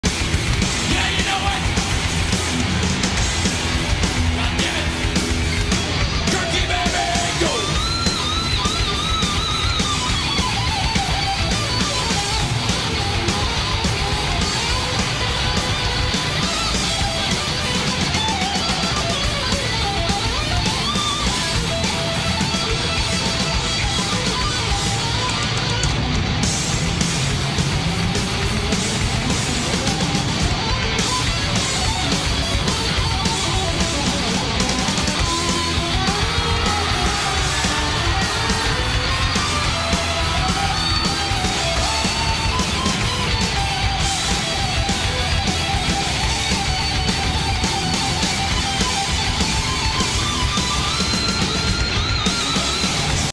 Kirk shreds like a monster!!!!
Metallica play at Moscow 1991!